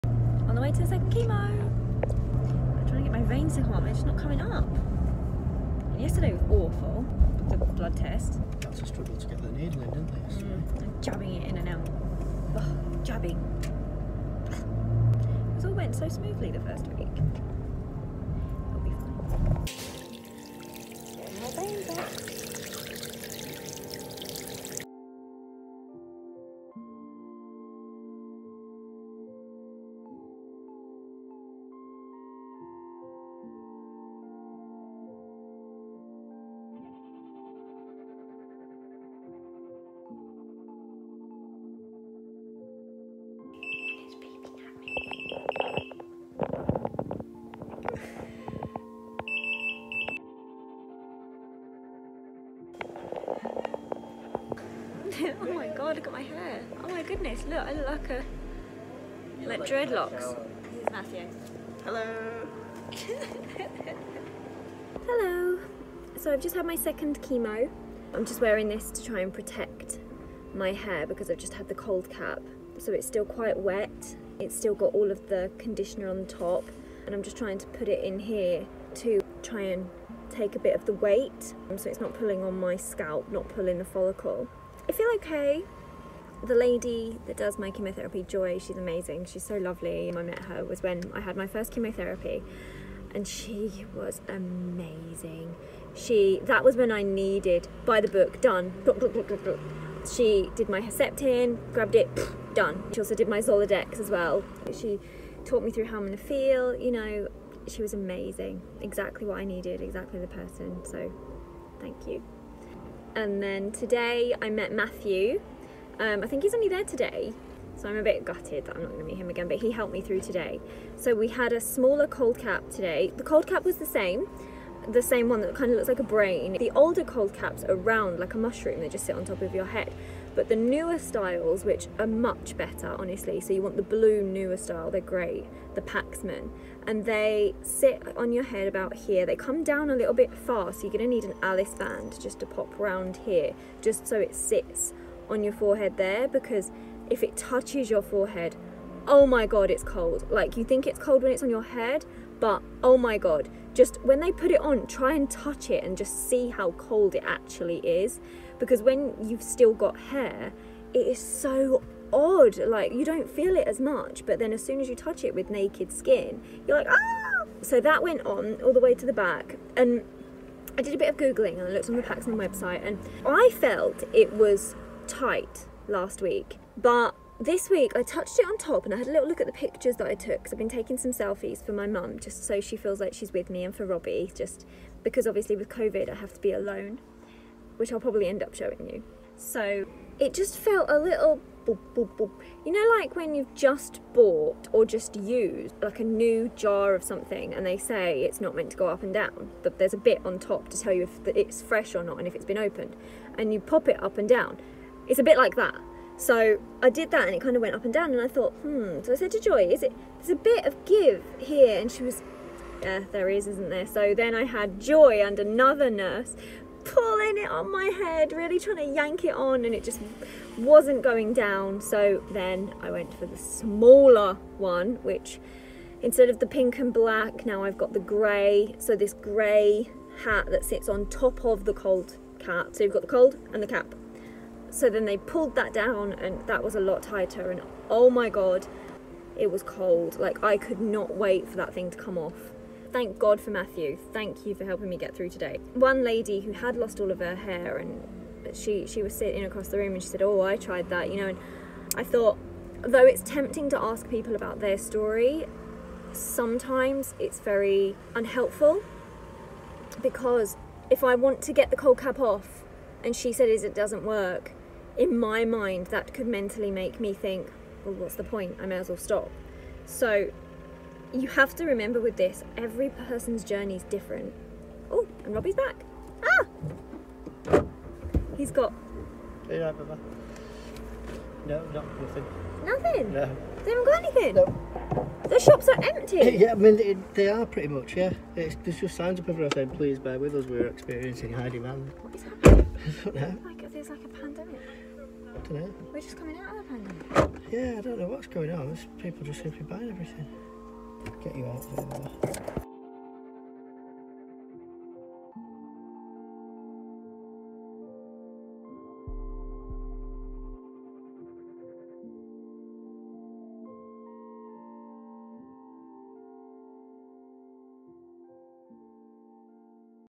My Cancer Journey - My Second Chemotherapy (Week Two vlog) Cold Cap + Mini vlog
Come with me to my second chemotherapy. I quickly learnt that no two weeks were the same, they each presented new challenges (this week my veins weren't playing ball). But don't get me wrong, there may have been challenges but the nurses kept me smiling!

00:20 At chemo
00:43 Beeping! *
01:05 Car vlog
03:19 Why did I change my cold cap?

* It's normal for the machine to beep, it just means that the medication is at an end or that the cannula needs to be checked.

𝕋𝔸𝔾𝕊 ➾
🎼 Music ➾ Created and owned by the creator 🎧